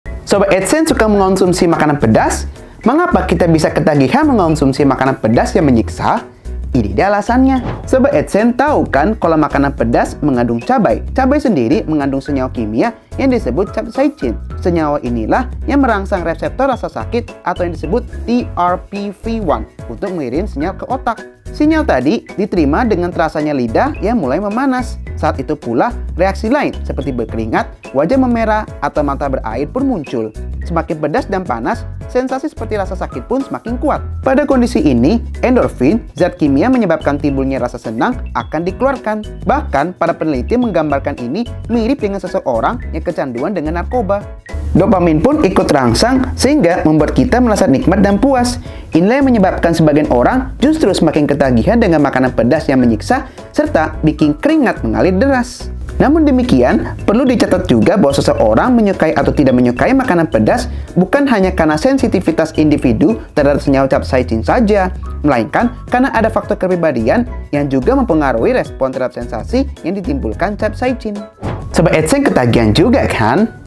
Pedas tapi nagih, capsaicin pada cabai bisa bikin bahagia
Ketika tubuh kita merasakan capsaicin, maka tubuh akan melepaskan zat kimia lain yang membuat kita merasa senang dan bahagia. Karena itu makanan pedas membuat kita ketagihan karena menimbulkan efek bahagia. Apa kamu suka makan makanan pedas?